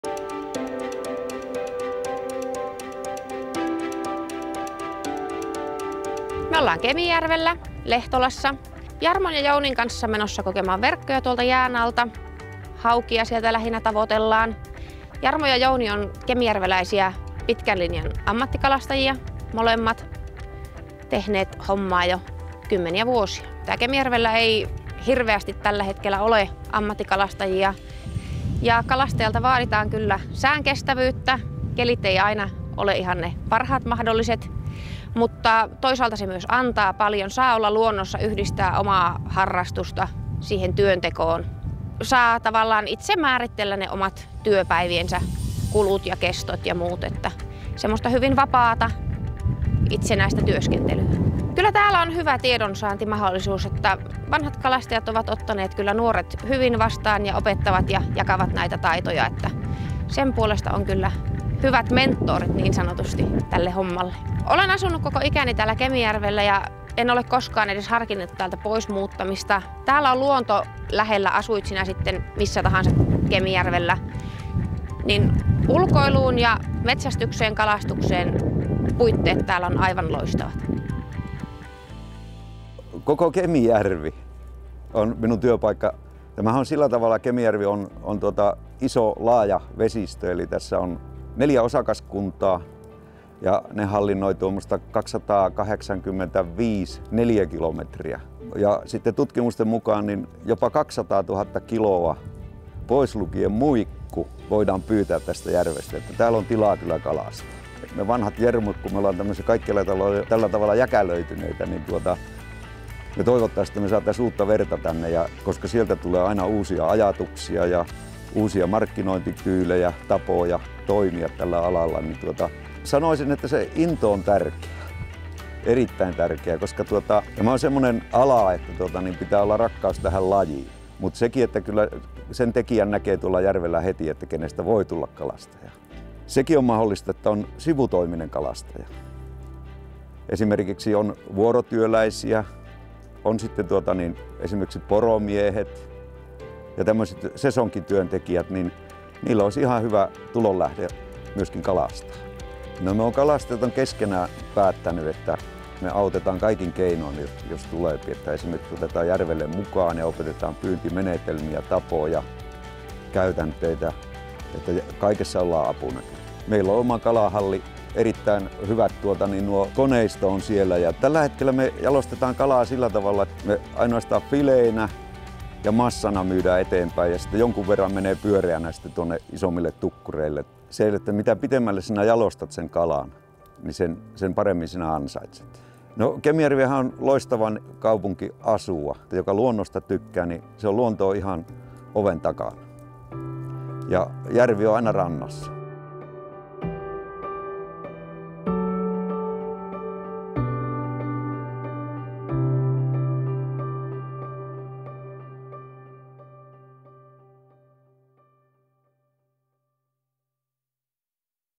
Kaupallisia kalastajia Kemijärvelle-hanke | Kemijärvi - Lapin järvimailla
AMMATTIKALASTAJAKSI LAPPIIN - KEMIJÄRVELLE

Haluamme turvata kaupallisen kalastuksen jatkuvuuden ja kehittymisen Kemijärvellä. Käytännön tavoite on saada Kemijärvelle uusia nuoria kalastajia tiedottamalla alueen tarjoamista hyvistä mahdollisuuksista harjoittaa kaupallista kalastusta. Hankkeella pyritään sekä herättämään nuorten (alle 40 v.) kiinnostusta kalastajan ammattia kohtaan, että kertomaan valtakunnan laajuisesti Kemijärvestä kalastajan työpaikkana.